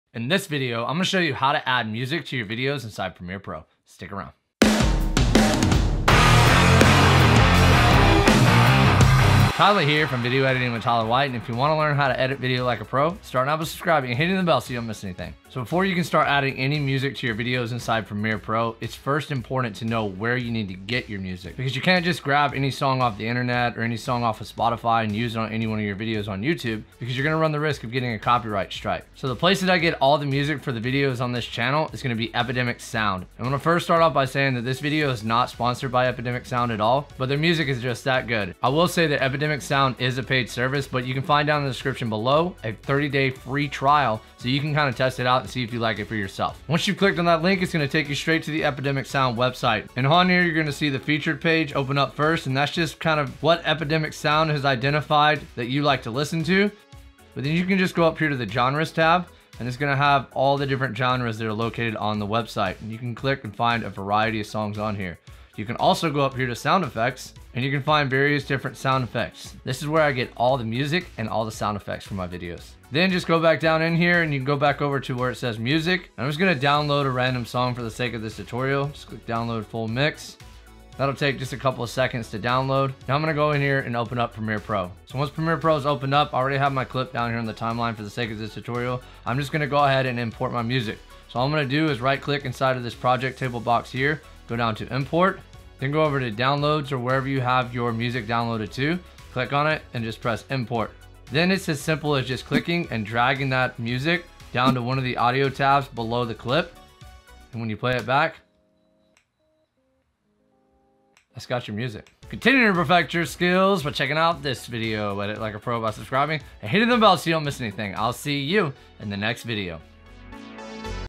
How to Add Music to Your Video Adobe Premiere Pro 2021
In this video, I show you exactly how to add music to your videos in Premiere Pro. Music is important when it comes to storytelling and can really make your edits come to life. If you’re uncertain about how to add music to video in Premiere Pro you’ve come to the right place. This is also how to add background music in Adobe Premiere Pro. By the end of this video, you’ll know how to add music in Premiere Pro 2021, 2020 and many other versions of Adobe Premiere Pro!

Check out
📙 My FREE Video Editing e-book

Another Great Place Where I Get the Music for My Videos↙